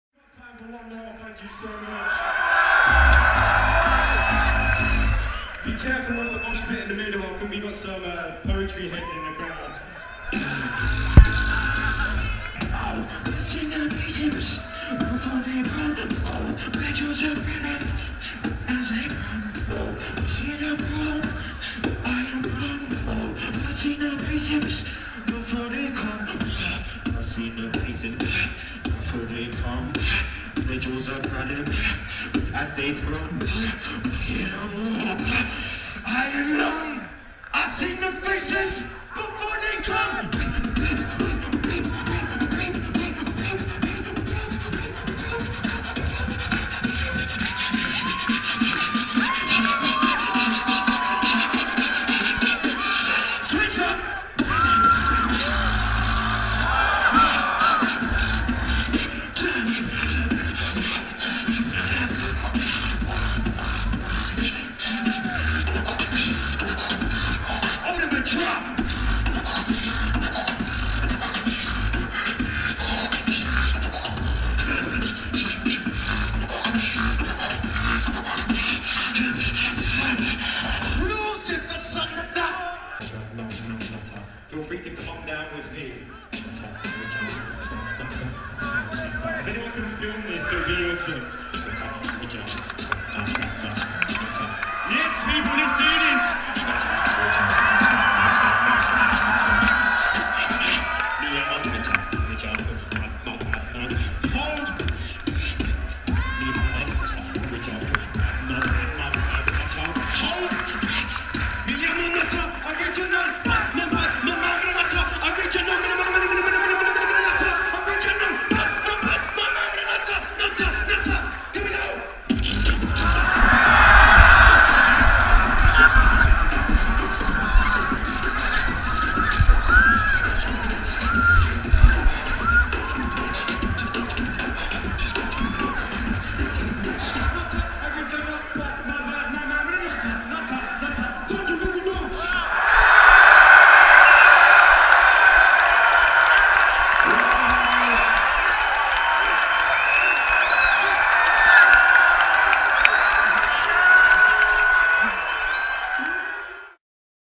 Reeps One | Reading Festival 2014 | Pit Cam
not my normal style of music, this is by far away from the music spectrum my fanbase enjoy but ive been a big fan of reeps for a while and this set kinda set off, reeps one is an awesome showman and graffiti writer, big up reeps one.